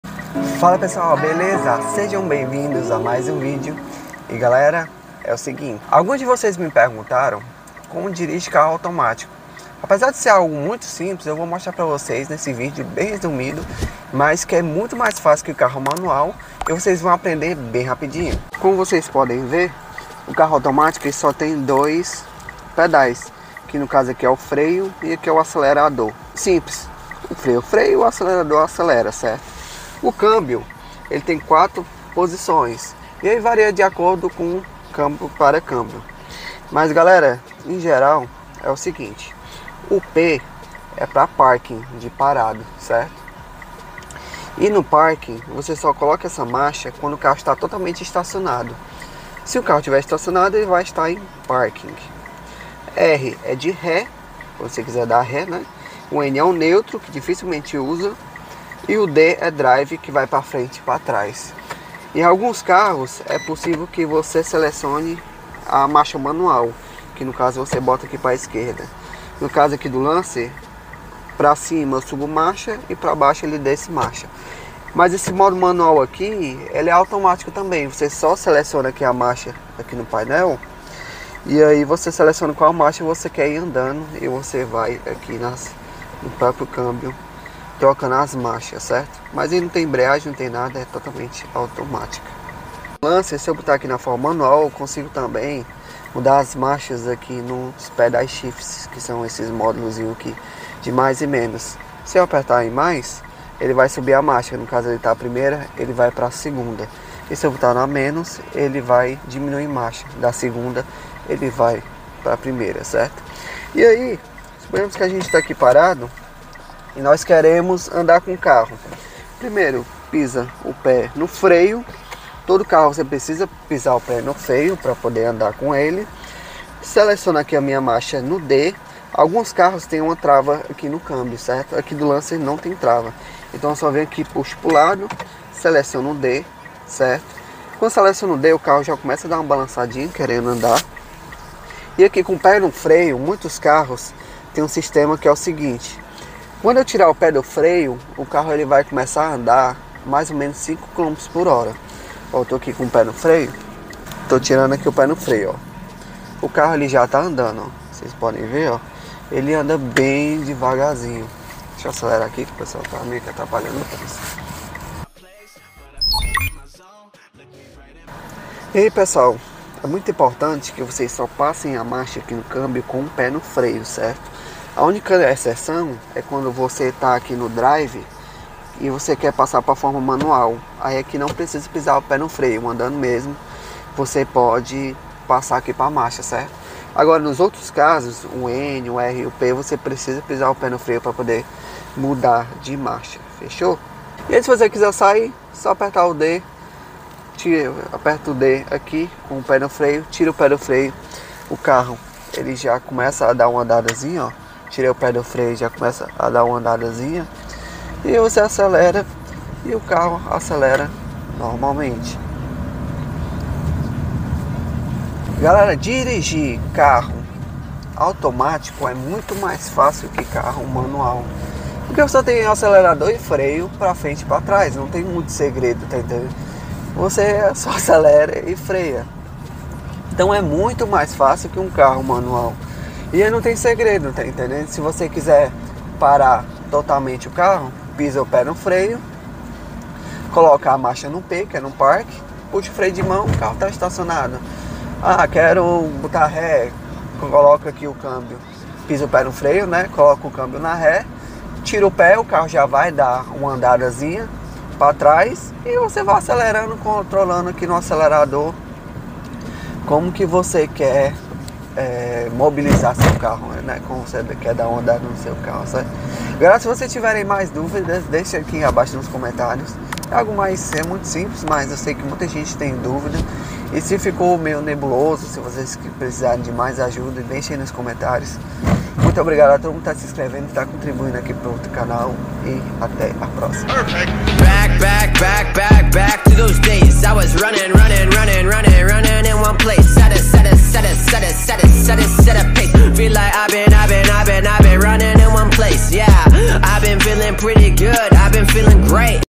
COMO DIRIGIR CARRO AUTOMÁTICO RÁPIDO E FÁCIL | O GUIA ABSOLUTAMENTE COMPLETO
Dirigir carro manual é um processo que todos aprendem ao fazer a carteira de motorista, mas carro automático a autoescola não ensina e acabamos aprendendo na marra mesmo! Apesar de algumas pessoas acharem de que manual é mais fácil que automático neste vídeo eu te mostro que é totalmente o inverso. É possível aprender a dirigir carro automático em menos de 5 MINUTOS!
É muito simples e RÁPIDO de APRENDER